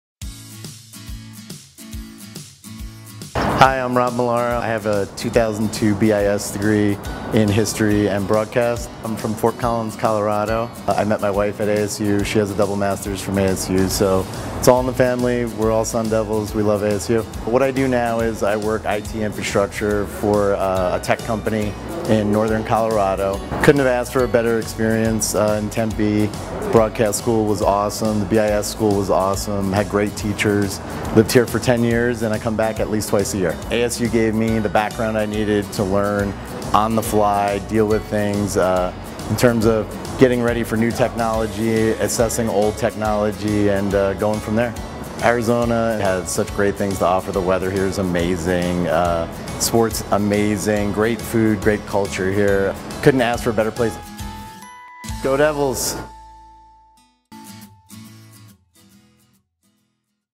Why I Came to ASU: Grads from Colorado | Arizona State University
An Arizona State University alumnus from Fort Collins, Colorado tells us why he is happy with his decision to come to ASU.

Rob graduated in 2002 with a degree in History and Broadcast. He met his wife at ASU, who has a double masters, also from Arizona State University. (Go Devils!)

Rob currently works IT infrastructure for a tech company. He loved his experience so much that he comes back to visit Arizona twice a year.  He says ASU gave him the background he needed to learn on the fly so he can easily keep up with the changing technology.